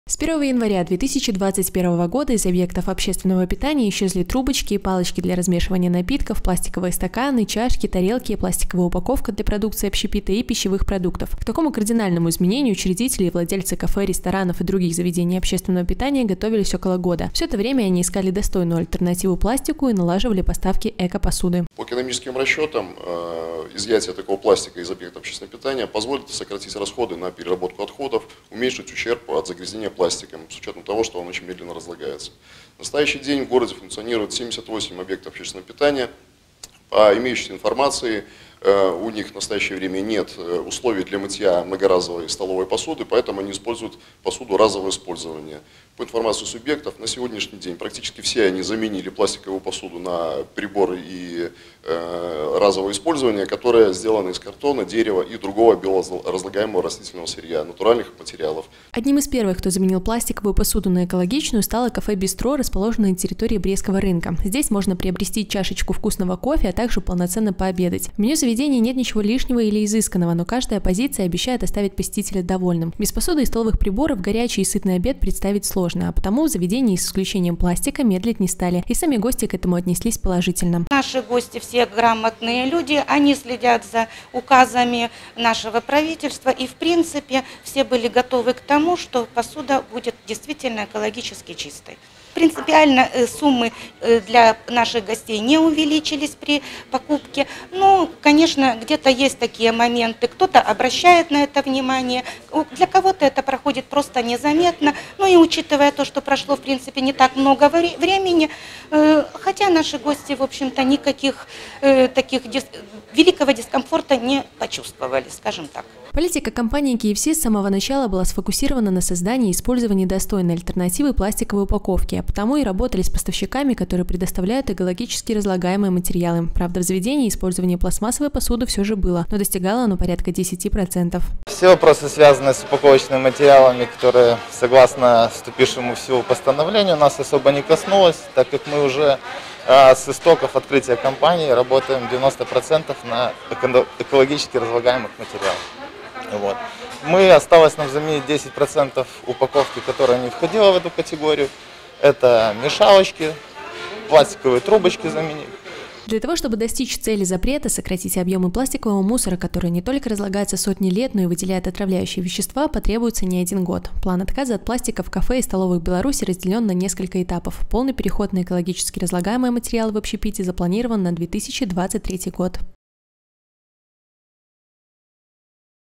2021-02-04 г. Брест. Экологичный тренд-новый стиль жизни общепита. Новости на Буг-ТВ.
Экологичный тренд, для многих  стал стилем жизни. Ходить в магазин со своей сумкой, не класть продукты в полиэтиленовые пакеты, а также отказаться от приобретения пластиковой посуды – простые, но такие полезные привычки, которые внедряют в свою жизнь брестчане. Быть эко-френдли, что буквально означает «дружественный   экологии», горожанам помогают и точки общественного питания. В новом году все владельцы и учредители брестских кафе, ресторанов и других заведений общепита, сказали свое твердое «нет!» пластику. Какие плюсы извлекают из нововведения, узнавали наши корреспонденты.